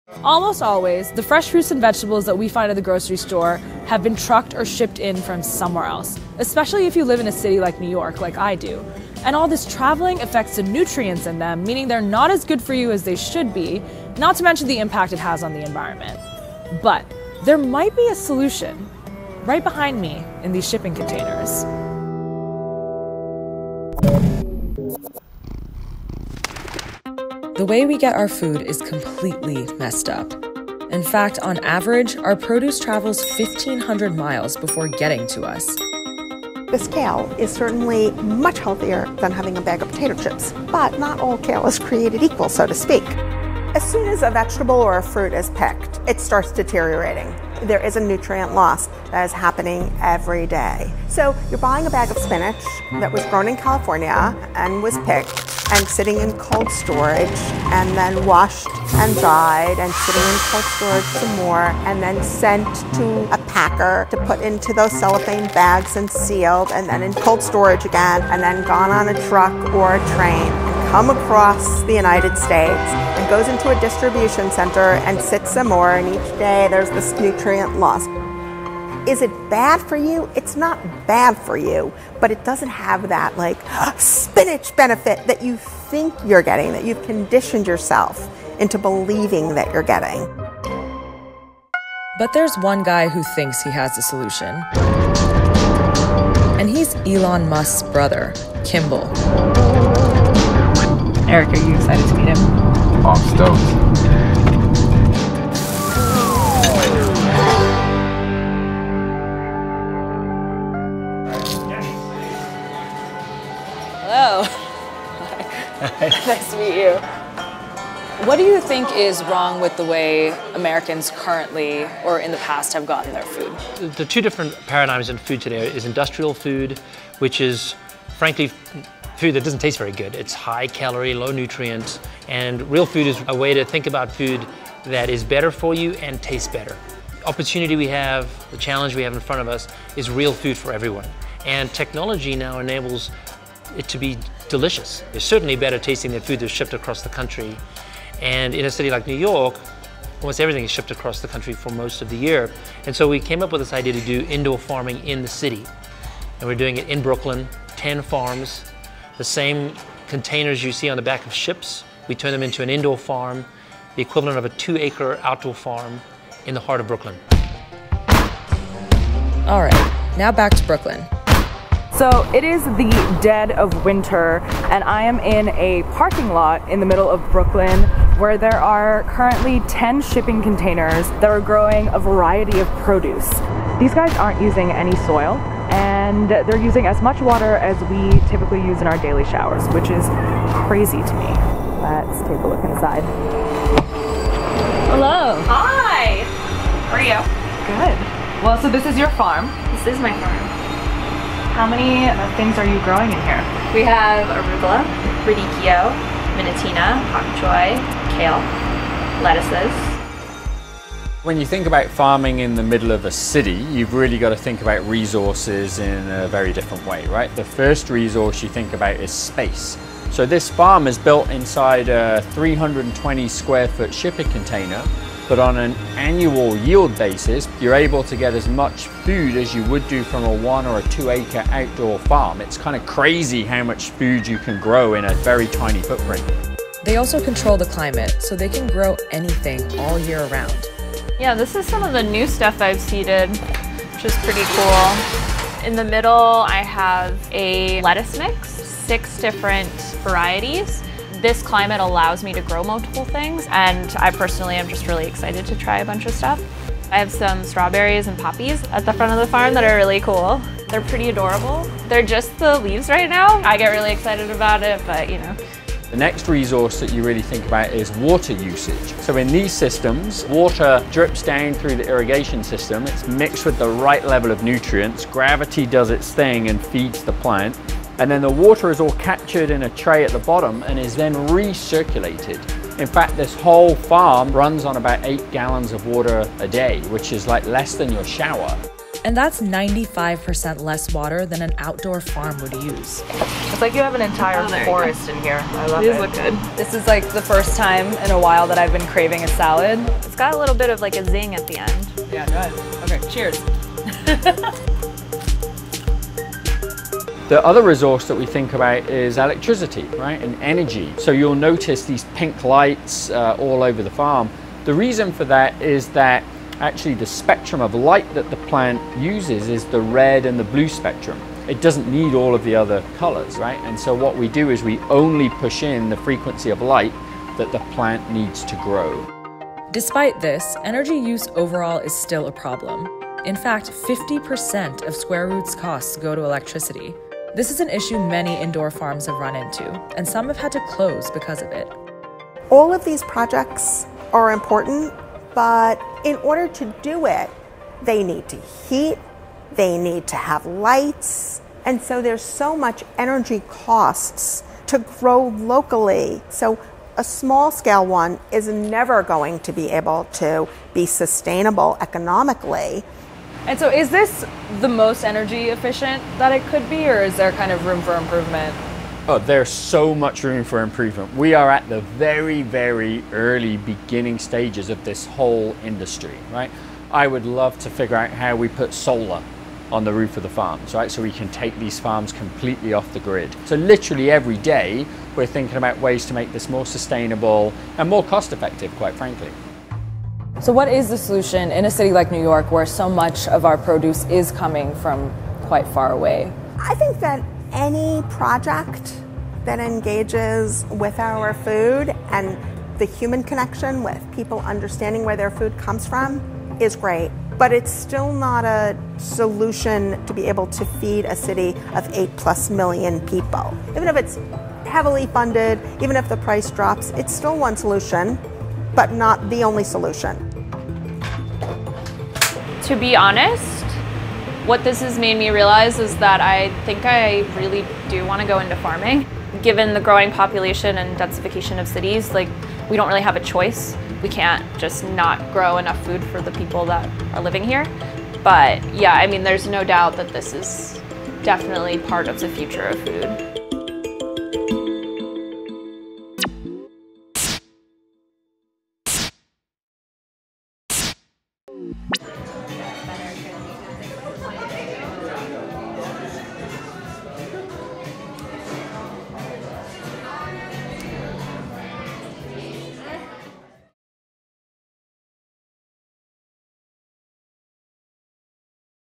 Kimbal Musk's Farm of the Future (Yes, Elon's brother)
The way we get our food in the United States is completely messed up. When you go to the grocery store, especially in a city, most of the fresh fruits and vegetables you see have been trucked in from somewhere else, losing crucial nutritional value at an environmental cost. This is a huge problem, but Elon Musk’s brother has a solution...in the form of a shipping container.

Kimbal Musk has a Silicon Valley - Style Plan to Feed America

Why Kimbal Musk Is a Founding Father of the Modern Food

The Vertical Farm: Growing crops in the city, without soil or natural light -